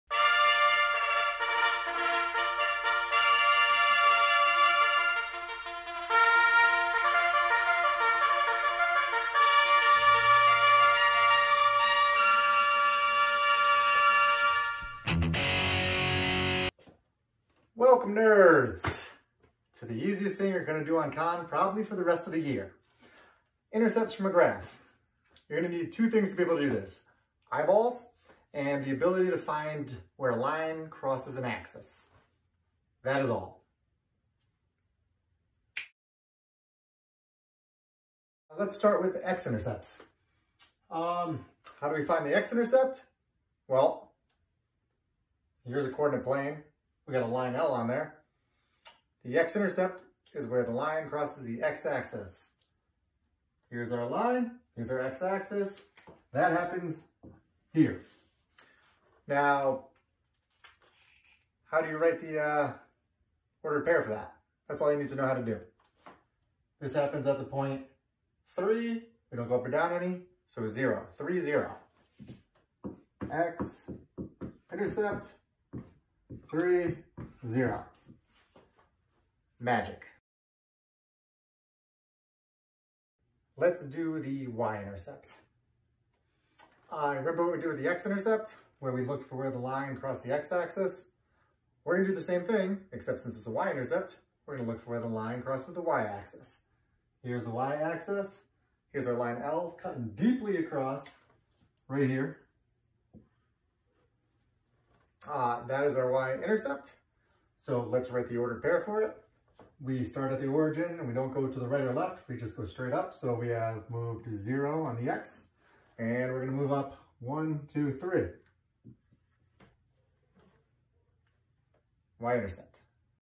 Intercepts from a Graph - DLP Week 4
Video showing how to write the ordered pairs for x and y-intercepts from a graph